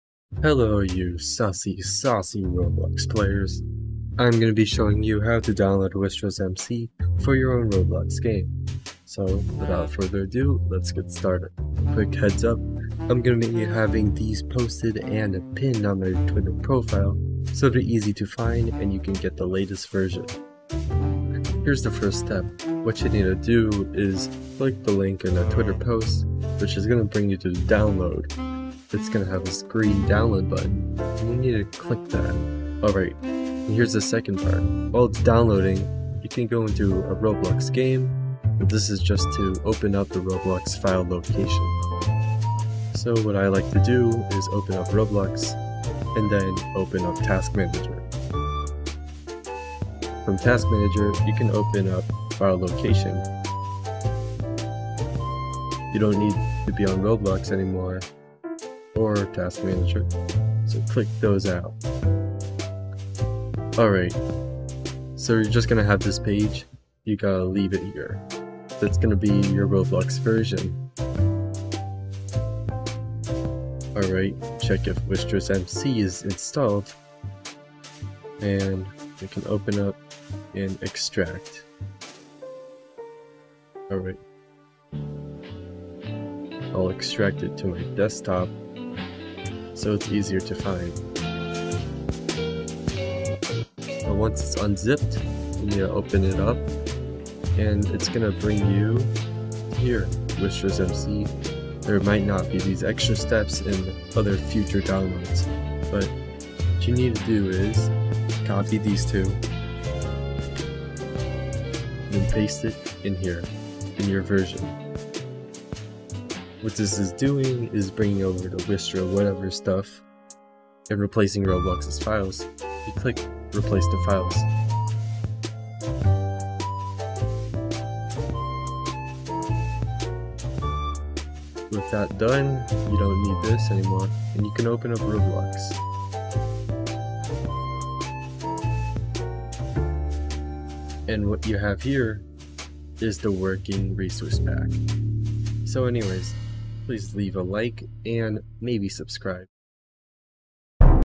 Roblox Resource Pack (Wistra's MC) Download Guide ~ (UI only)
Since this was highly requested, I made an installation guide!
I hope this one helps a lot and clears up much confusion regarding installation, if not, I have a tutorial I made a while back posted somewhere on my twitter